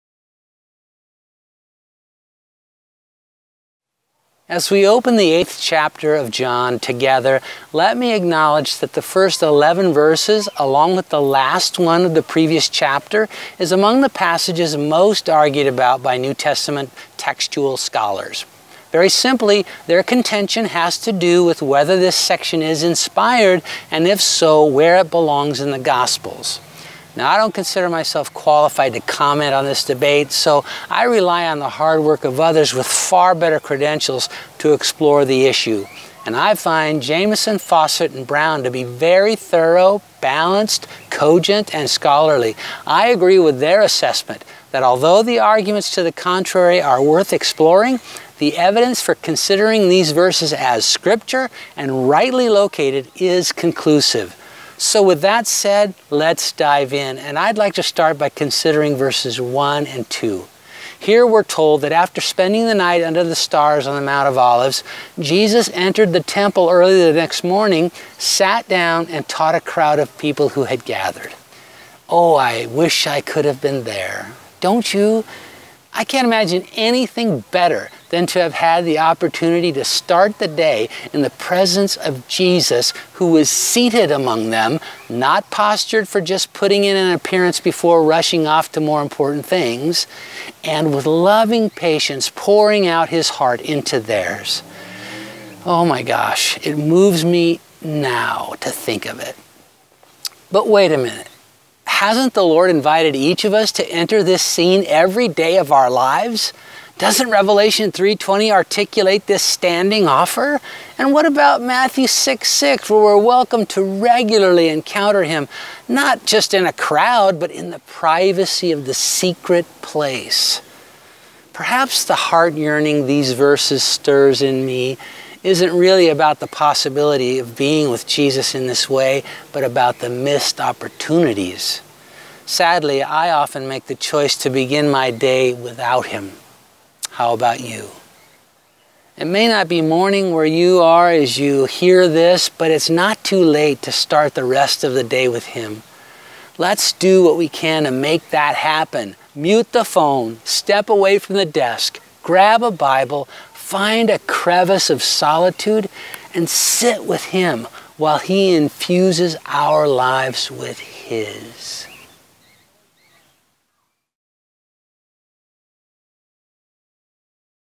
John 8:1-2 • Missed Opportunities
As we open the eighth chapter of John together, let me acknowledge that the first 11 verses along with the last one of the previous chapter is among the passages most argued about by New Testament textual scholars. Very simply, their contention has to do with whether this section is inspired, and if so, where it belongs in the Gospels.

Now, I don’t consider myself qualified to comment on this debate. So, I rely on the hard work of others with far better credentials to explore the issue. And I find Jameison, Fausset, and Brown to be very thorough, balanced, cogent, and scholarly. I agree with their assessment that although the arguments to the contrary are worth exploring, the evidence for considering these verses as Scripture and rightly located is conclusive.

So, with that said, let’s dive in. And I’d like to start by considering verses 1 and 2. Here we’re told that after spending the night under the stars on the Mount of Olives, Jesus entered the Temple early the next morning, sat down, and taught a crowd of people who had gathered.

Oh, I wish I could have been there. Don’t you? I can’t imagine anything better than to have had the opportunity to start the day in the presence of Jesus, who was seated among them – not postured for just putting in an appearance before rushing off to more important things – and with loving patience, pouring out his heart into theirs. Oh my gosh! It moves me now to think of it.

But wait a minute. Hasn’t the Lord invited each of us to enter this scene every day of our lives? Doesn’t Revelation 3:20 articulate this standing offer? And what about Matthew 6:6 where we’re welcomed to regularly encounter him not just in a crowd but in the privacy of the secret place?

Perhaps the heart-yearning these verses stir in me isn’t really about the possibility of being with Jesus in this way but about the missed opportunities. Sadly, I often make the choice to begin my day without him. How about you?

It may not be morning where you are as you hear this, but it’s not too late to start the rest of the day with him. Let’s do what we can to make that happen. Mute the phone, step away from the desk, grab a Bible, find a crevasse of solitude, and sit with him while he infuses our lives with his.